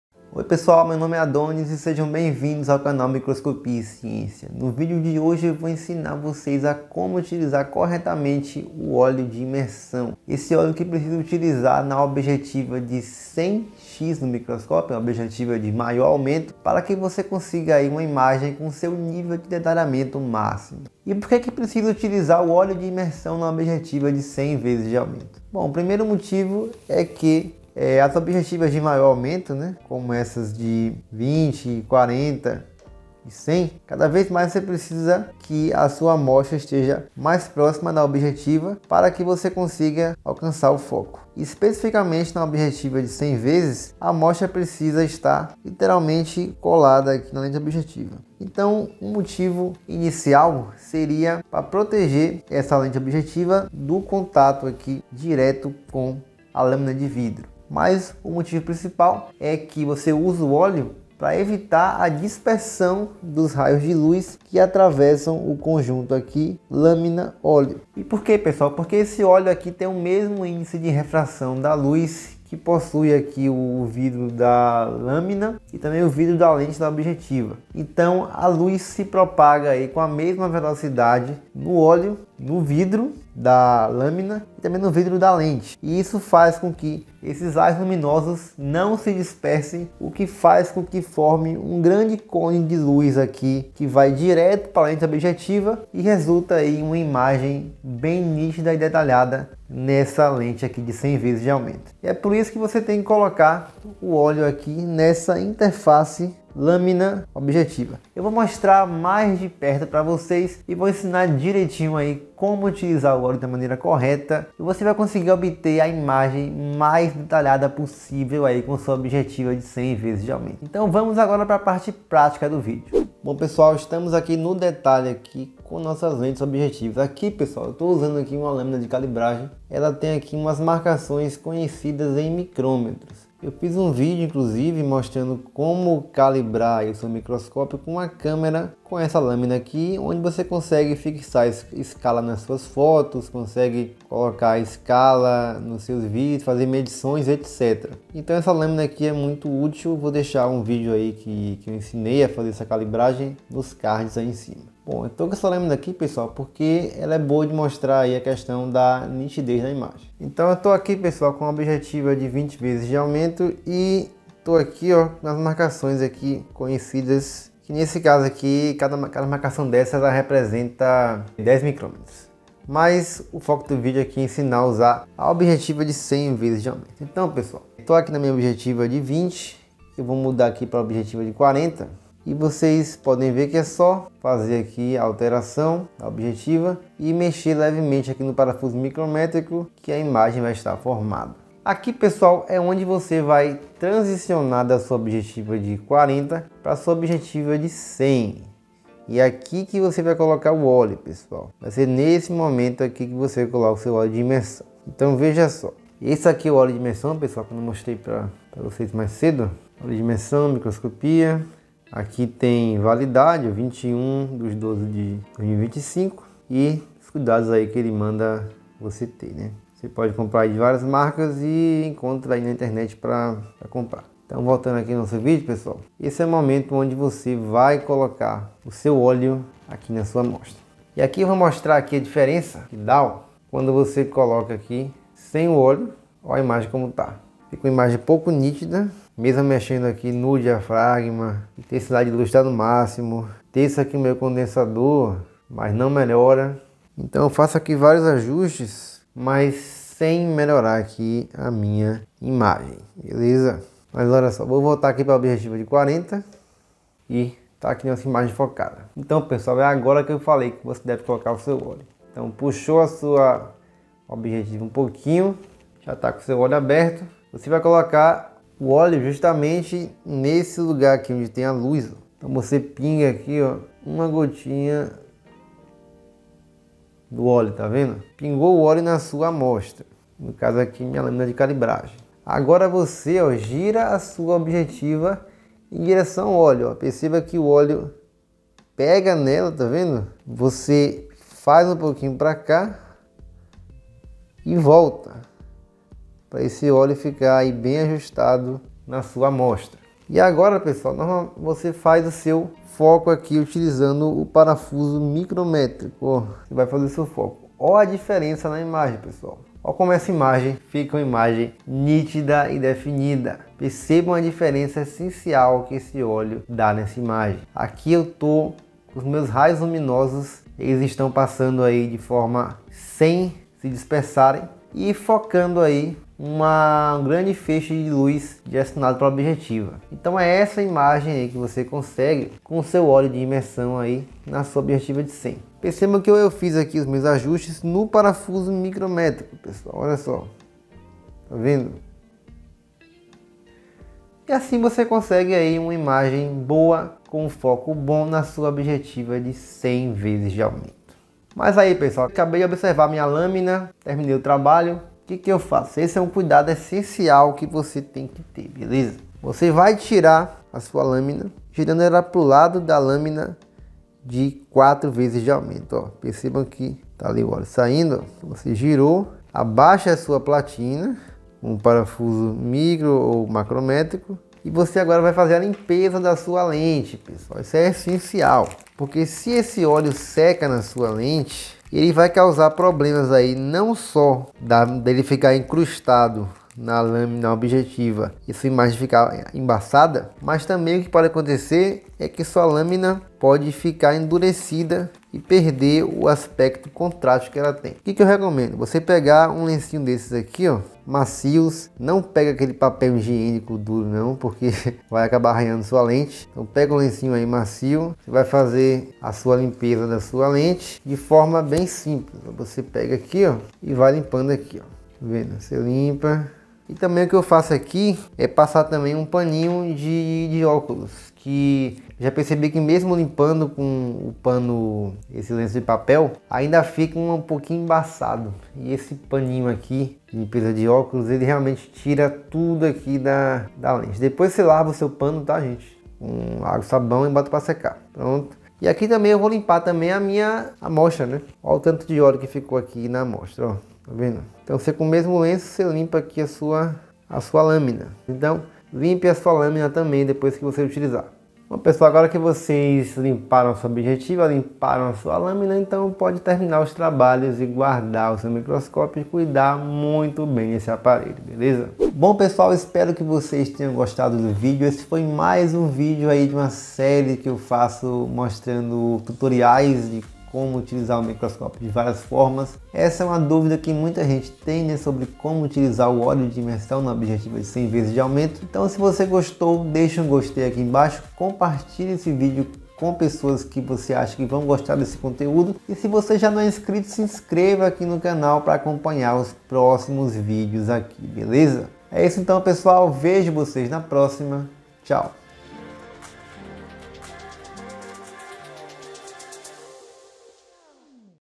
Como Usar Óleo de Imersão na Objetiva de 100x - Guia Completo!
Descubra o segredo para imagens microscópicas incrivelmente nítidas com esse tutorial completo sobre como usar óleo de imersão na lente objetiva de 100x! 🌟

Neste vídeo, explico detalhadamente porque utilizar óleo de imersão é essencial para alcançar o máximo de detalhamento de imagem na sua objetiva de 100x. Vou te guiar por cada etapa:

1 - Por que usar óleo de imersão? Explico a ciência por trás do uso deste óleo, e como ele ajuda a obter imagens mais claras e com maior resolução.

2 - Aplicação passo a passo: Mostro exatamente como aplicar o óleo na sua lente, garantindo que você alcance uma melhor imagem na objetiva de 100x

3 - Comparativo prático: Te mostro a diferença nas imagens com e sem o uso do óleo de imersão. Você ficará impressionado com o antes e depois!
Se você utiliza microscópios na escola, universidade, ou mesmo se é um entusiasta da microscopia em casa, este vídeo é perfeito para você.

Capítulos:
00:00 Introdução
02:03 Utilizando o óleo de imersão
05:52 Fazendo a limpeza de sua lente
07:43 Finalização do vídeo

GUIA  COMPLETO  de como calibrar um microscópio óptico para REALIZAR MEDIÇÕES E FIXAR ESCALA nas suas fotos: